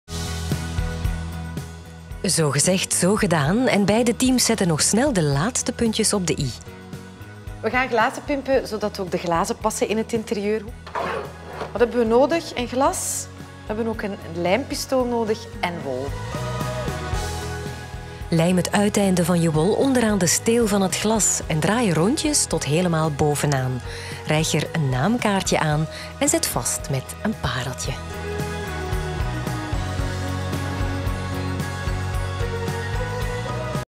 Versierde glazen met naamkaartje
Bij de babyborrel van Toeters & Bellen worden zelfs de glazen versierd door Elke Van Mello met wol en een naamkaartje.